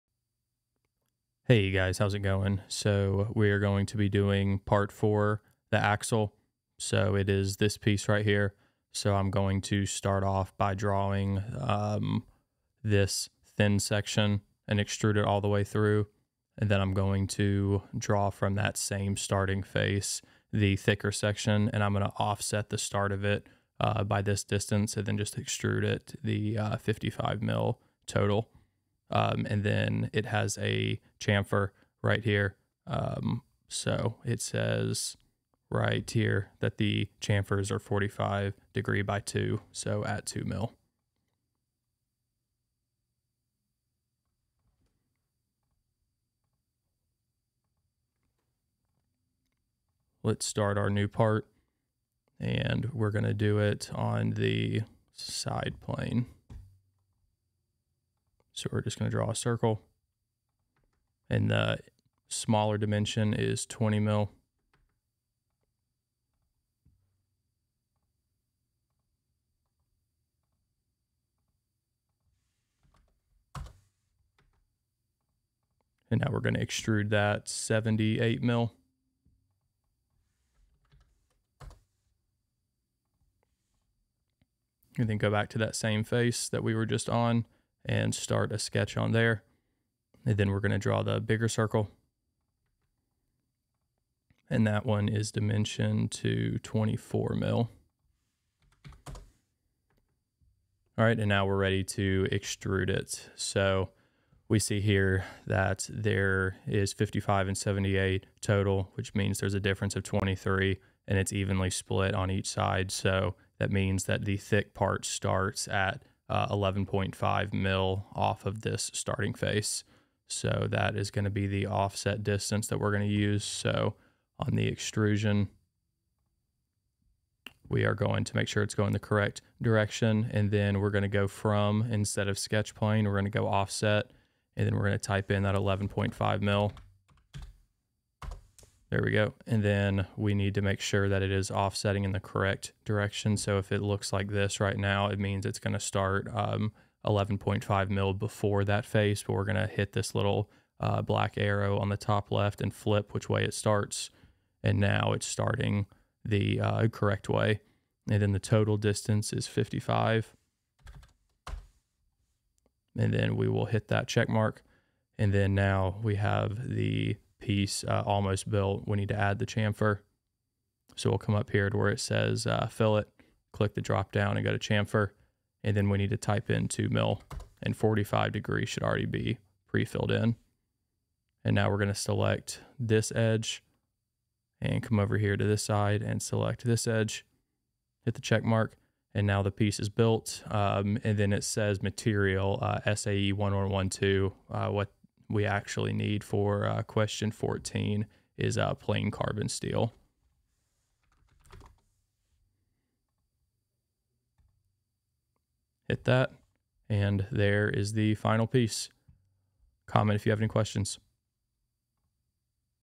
Chapter 14 Part 4 Axle SolidWorks Tutorial
This is a SolidWorks tutorial of making the Axle from Parametric Modeling with SolidWorks 2022 Chapter 14, Part 4.